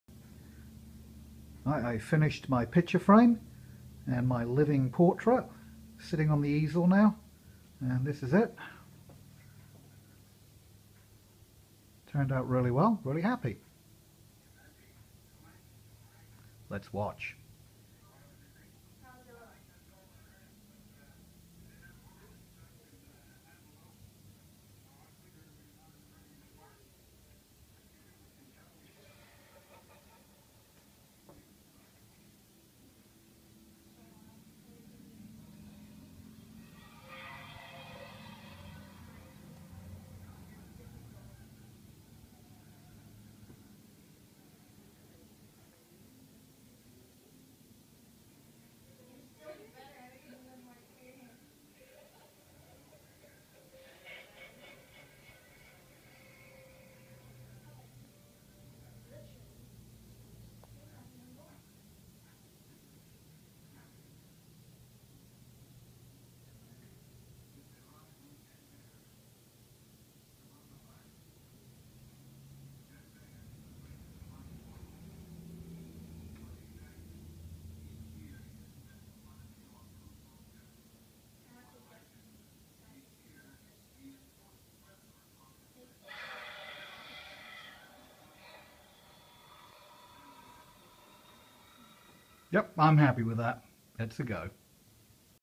Living Portrit
I used the digital FX from AtmosFearFX to create this prop. I built a picture frame and antiqued it which I installed a 19" LED TV I then built an easel to display it on. I am really happy how it turned out.